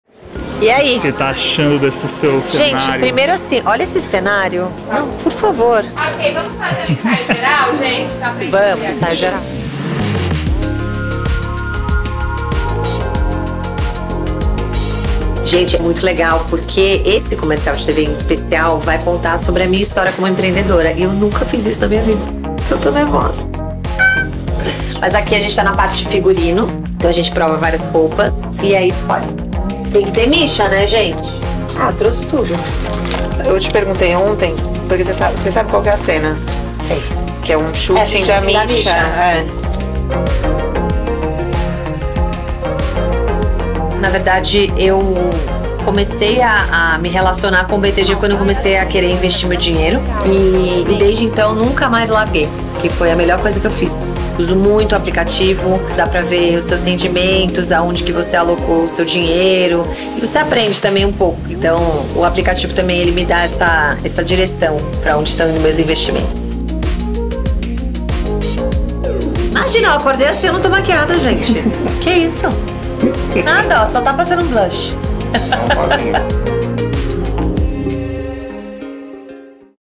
O que a Mica Rocha mais gosta do BTG Pactual?
Convidamos nossa cliente e parceira Mica Rocha para participar do novo vídeo do Banco e aproveitamos para conversar sobre como essa relação de confiança foi construída e como o BTG a ajudou a potencializar sua trajetória.

Acompanhe um pouco dos bastidores da gravação.